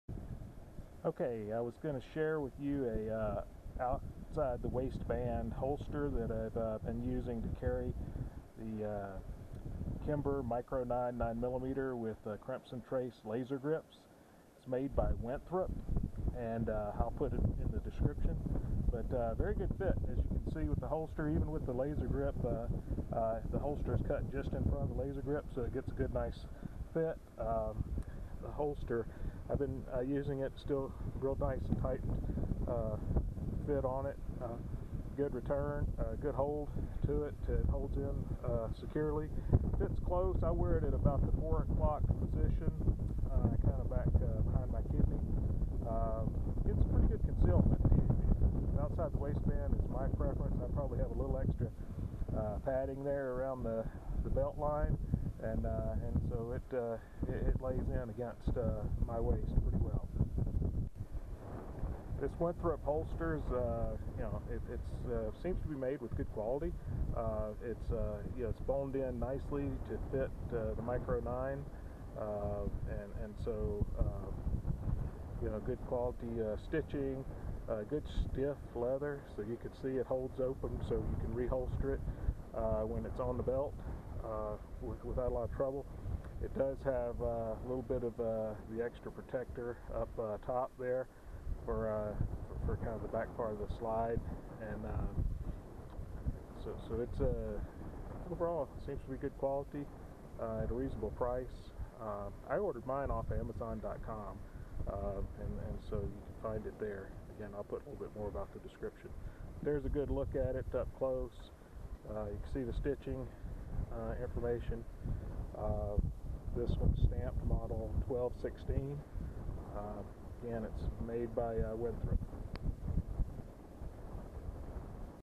Kimber Micro 9 - review of the Winthrop OWB Holster
Winthrop OWB holster review for Kimber Micro 9 with Crimson Trace laser grips. Very well constructed holster that fits the Kimber Micro 9 well, allowing for the laser grips.